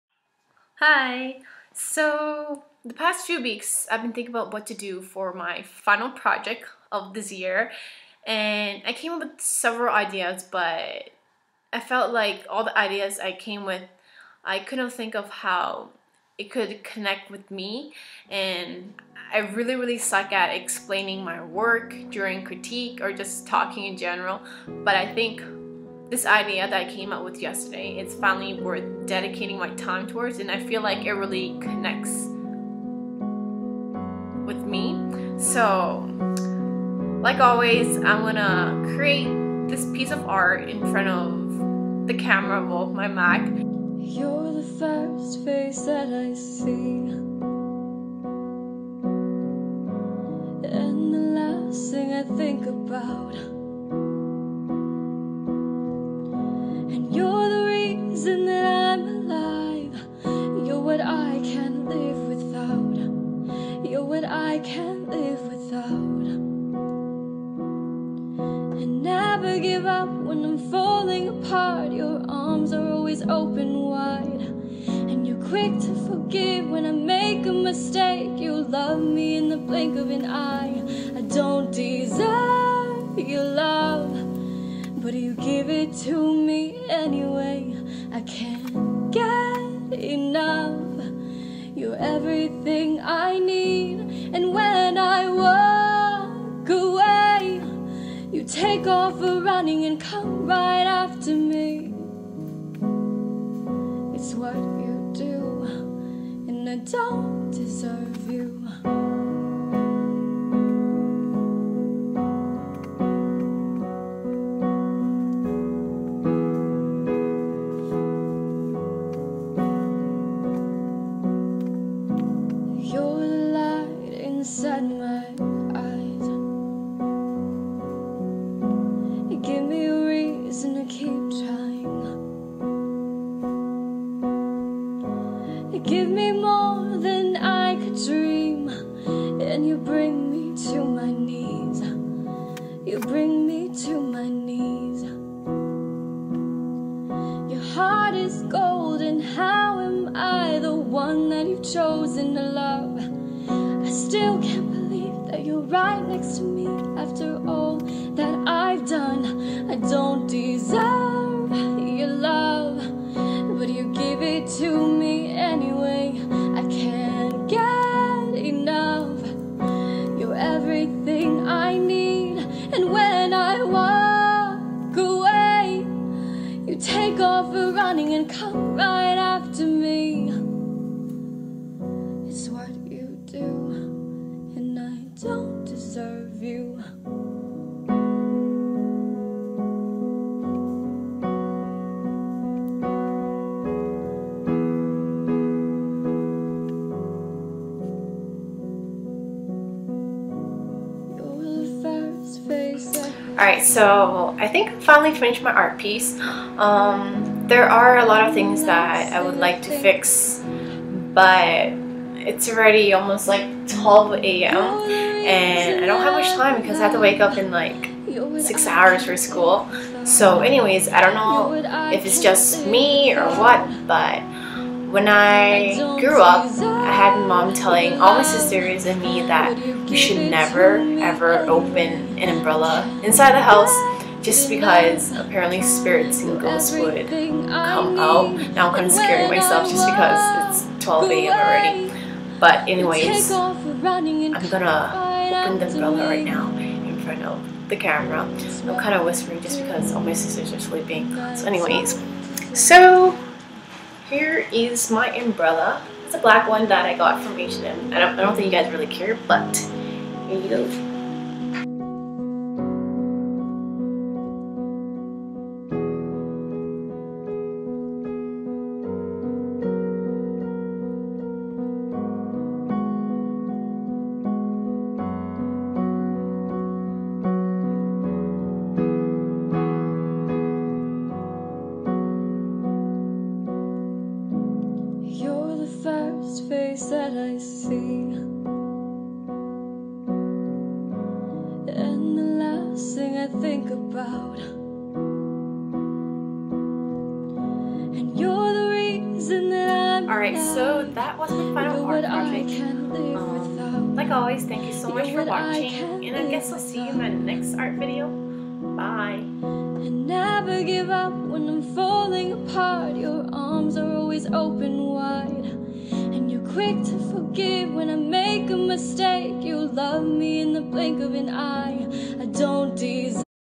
Cityscape Painting "Umbrella Art"
I love painting landscapes and I love portraying my art on different materials.

This was for my final project and I came up with this umbrella idea because I wanted something that would speak to the viewers (proff/classmates) about who I am as an individual. I am a quiet girl and I don't really open myself to others- I choose when to open and close myself.

As you can see, the painting isn't all that clear because I wanted to show how even when I open myself to people, they only have an idea of who I am, they don't know all the fine little details about me".

wanna see more of my art work?